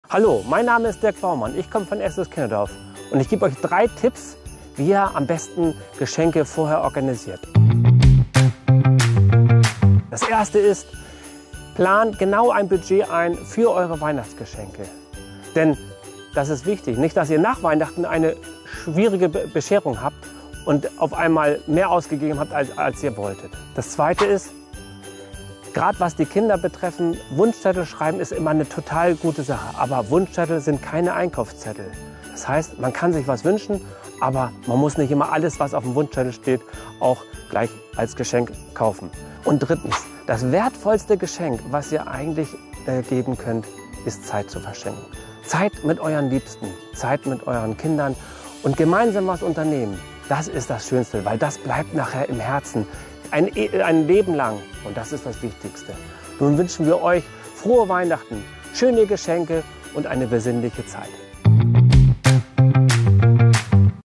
✨kurz erklärt✨ Teil 28: Weihnachtstipps Teil 2
Unser Pädagoge Dirk Baumann hat drei wertvolle Tipps fürs Besorgen von Weihnachtsgeschenken. Schaut rein in die neueste Folge und holt euch die Tipps.